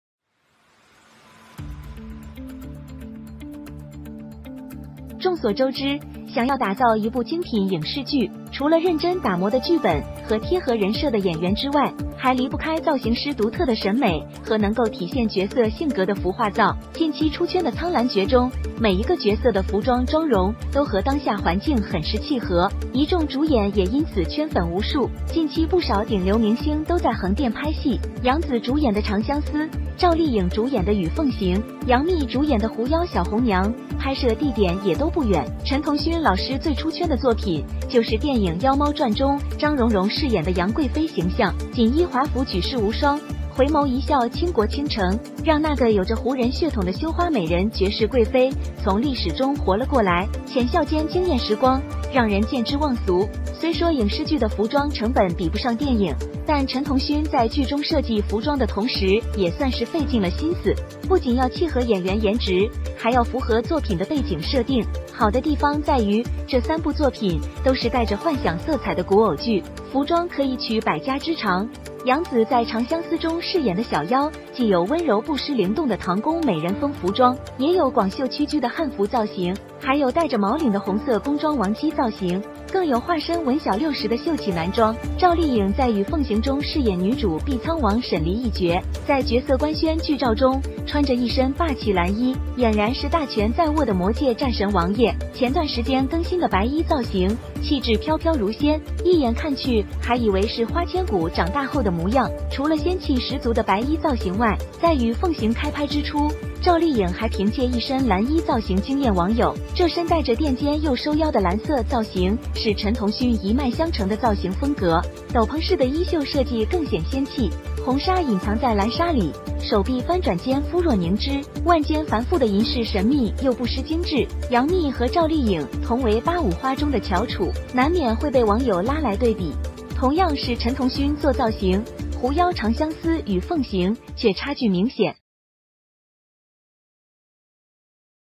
It is also the costume style made by Chen Tongxun, but the gap between Zhao Liying, Yang Mi and Yang
As we all know, in order to create a fine film and television drama, in addition to a carefully polished script and actors who fit the persona, it is also inseparable from the stylist's unique aesthetic and can reflect the character of the costume and make. In the recently released "Canglan trick", each character's costume and makeup are very suitable with the current environment, and a number of main actors and actresses are thus numerous. Recently many top stream stars are filming in Hengdian, Yang Zi starring in "Changxiang Si", Zhao Liying starring in "and Feng Xing", Yang Mi starring in "Fox Demon Little Red Queen" shooting location is not far. Coincidentally, the costume stylist for all three works is Chen Tongxun, whose works are the costumes for famous movies and TV dramas like "Legend of Harem Zhen Huan". The most outstanding work of Mr. Chen Tongxun is the image of Yang Guifei played by Zhang Yongrong in the movie "The Legend of the Demon Cat", which is unparalleled in the world with its beautiful clothes and a smile. The most important thing is that the beauty of the flower of the Hu people's blood, the most beautiful consort from the history came to life, light smile between the stunning time, let people see the forgettable. Although the costume cost of film and television dramas is not as expensive as movies, but Chen Tongxun in the drama design costumes at the same time is also considered a great deal of thought, not only to fit the face of the actors, but also to meet the background of the work set. The good thing is that all three works are ancient puppet dramas with fantasy overtones, and the costumes can take the best of all worlds. Yang Zi's Xiao Yao in "Chang Xiang Si" has a gentle and dynamic Tang palace beauty style costume, a wide sleeve curved train Chinese costume, a red palace dress with a fur collar, and a beautiful male costume in the guise of Min Xiao Liu. With the change of costume modeling, Xiao Yao also in the gentle girl and domineering Wang Ji switch freely, so people are looking forward to this 1v3 immortal heroine drama. Zhao Li Ying in the "and phoenix line" in the role of the heroine Bi Cang Wang Shen Li, in the role of the official stills wearing a domineering blue dress, as if it is a great power in the hands of the king of war in the magic world. A while ago the updated white look temperament floating like immortality, a glance also thought that is the appearance of the flower thousand bones after growing up. In a white dress combed with medium bangs Zhao Liying perfectly interpreted the phrase "the more vegetarian you wear, the more immortal your temperament", the clothes swaying between the confident and calm, both the leisurely retired idyllic, but also the taste of the last century retro martial arts film. In addition to the fairy-like white look, in the beginning of the shooting of "and phoenix line", Zhao Liying also stunned netizens with a blue look. This body with padded shoulders and waistline blue look is Chen Tongxun's lineage of styling style, cape-style sleeves design more fairy, red veil hidden in the blue veil, arm flip between the skin like fat, the wrist complex silver jewelry mysterious and not lose exquisite. Yang Mi and Zhao Liying are also the best of the 85 flowers, and will inevitably be pulled by netizens to compare. More coincidentally, Yang Mi's new play "Fox Demon Little Red Queen" red sarong modeling and Shen Li's blue dress modeling general structure is almost the same, but Tushan Hong Hong's red dress is more demonic, waist design and no belt decoration, there is a kind of body into the natural stunning. Because Yang Mi as Tushan Hong Hong persona is a fox demon, so from the modeling to makeup are on the demonic side, as the Chosen fox demon Yang Mi eyes with love and not lose dominance, just from the road map to see people can not wait. Zhao Liying as the King of the Bicang Shen Li is a demon, but the immortal and earthly scenes are not small, the shape is more chivalrous and clear. Although these three works almost the same period of time to start, but also Chen Tongxun as stylist, but because of the shooting method and the content of the work is different, the final presentation of the work also differs significantly. If there is no accident, these three dramas will meet with you next year, are you ready to catch up with the drama? The same Chen Tongxun do styling, "fox demon", "long love", "and phoenix line" but the gap is obvious!